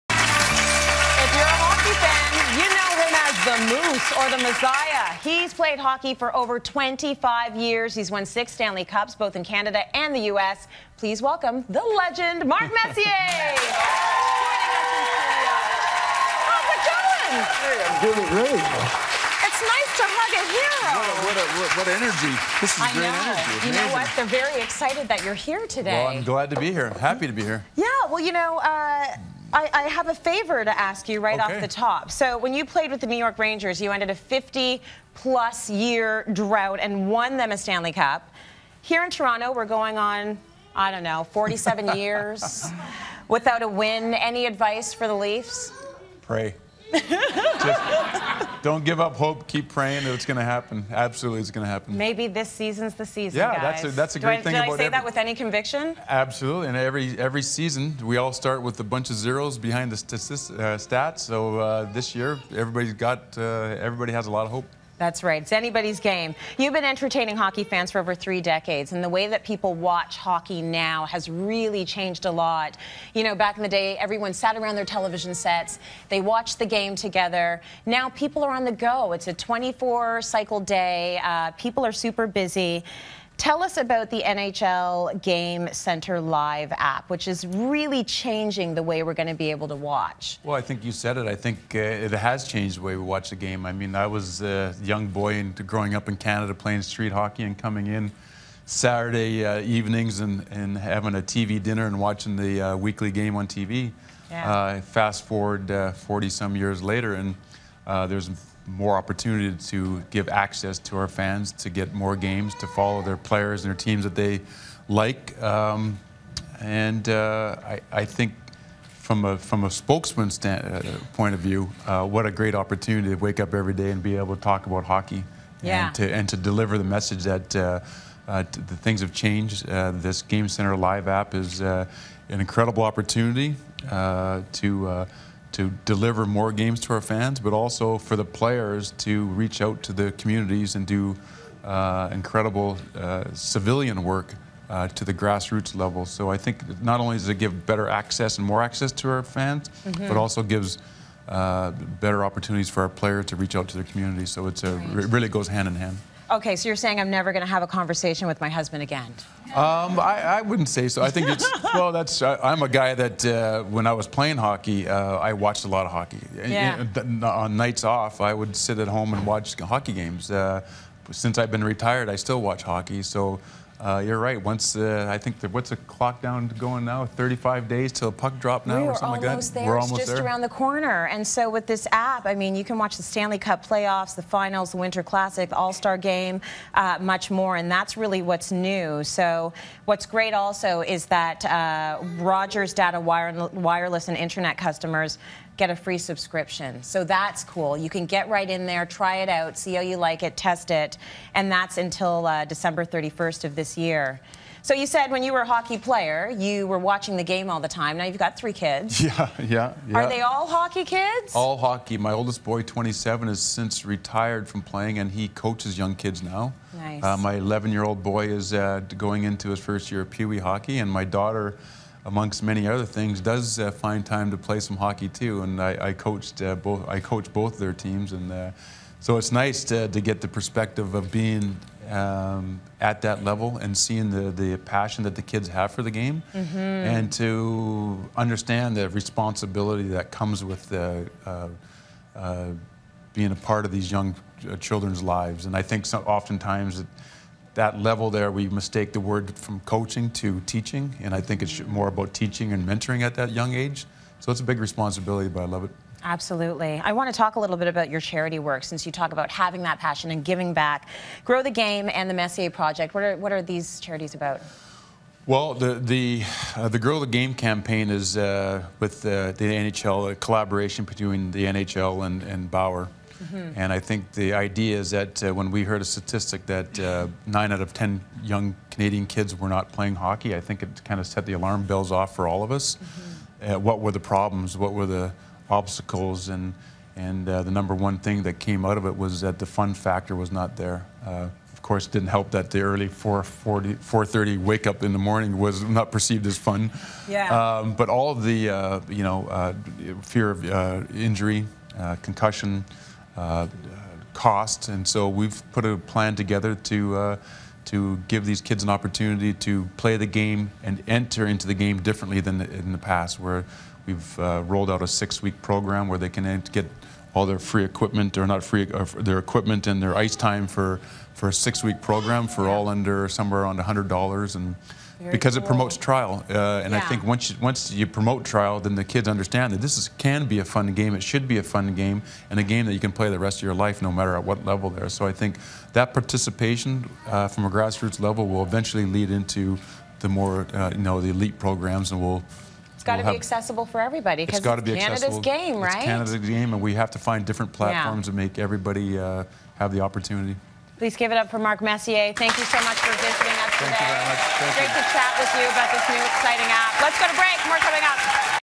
Mark Messier presents hockey’s new favourite app
One of the NHL’s greatest players sits down with us to discuss the new must-have app for all hockey fans, Rogers GameCentre LIVE.